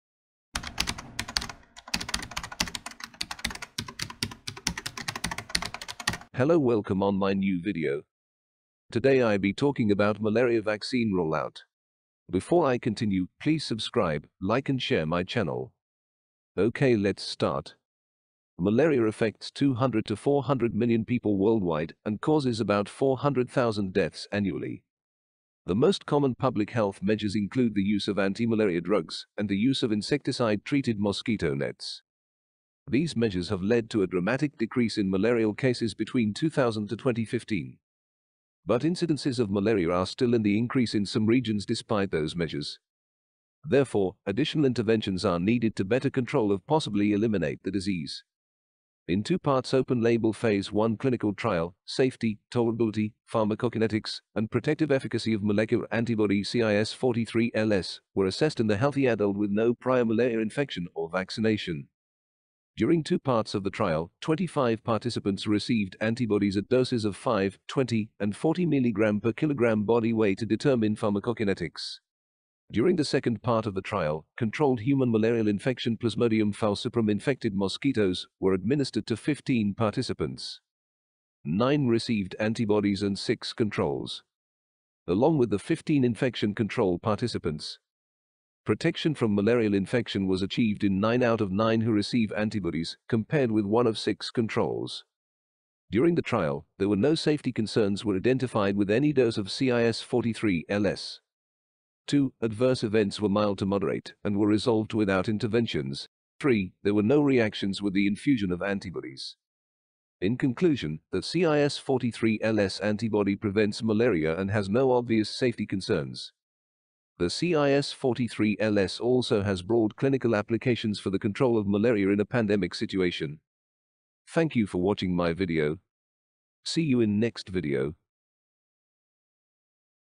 What You should know about Malarial Vaccine
Kenyan researchers celebrate WHO approval of new malaria vaccine